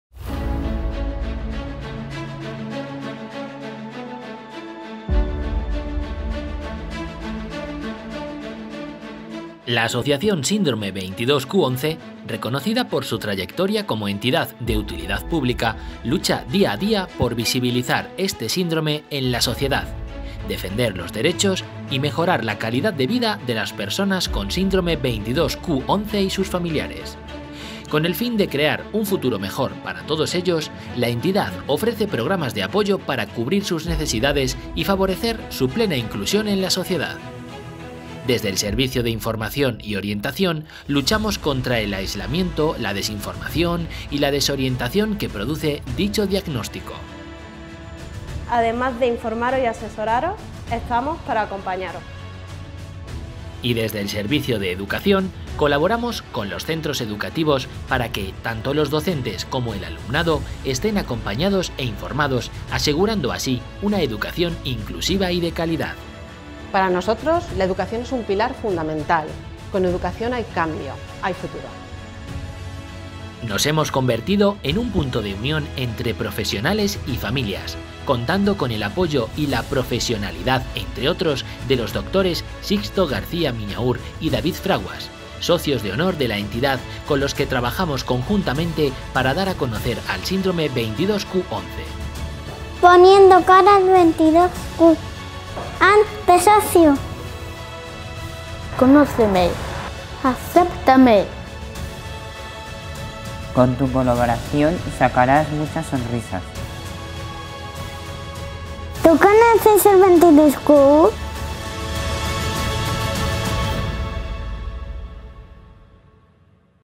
Corporativo - Asociación Síndrome 22Q11
La Asociación 22q España - 22q11.2, VCFS, DiGeorge lucha día a día por visibilizar este síndrome en la sociedad, defendiendo los derechos y mejorando así la calidad de vida de las personas con Síndrome 22q11 y sus familiares 😍
Nosotros, desde hace unos meses, 𝐭𝐞𝐧𝐞𝐦𝐨𝐬 𝐥𝐚 𝐬𝐮𝐞𝐫𝐭𝐞 𝐝𝐞 𝐡𝐚𝐛𝐞𝐫 𝐜𝐨𝐦𝐞𝐧𝐳𝐚𝐝𝐨 𝐚 𝐭𝐫𝐚𝐛𝐚𝐣𝐚𝐫 𝐣𝐮𝐧𝐭𝐨 𝐚 𝐞𝐥𝐥@𝐬 🎬 y no os vamos a engañar, estamos encantados 😊
Os dejamos el primero de muchos proyectos que esperamos producir a su lado 📹

𝘊𝘰𝘯𝘷𝘪𝘦𝘳𝘵𝘦 𝘭𝘰 𝘪𝘮𝘱𝘰𝘳𝘵𝘢𝘯𝘵𝘦 𝘦𝘯 𝘦𝘵𝘦𝘳𝘯𝘰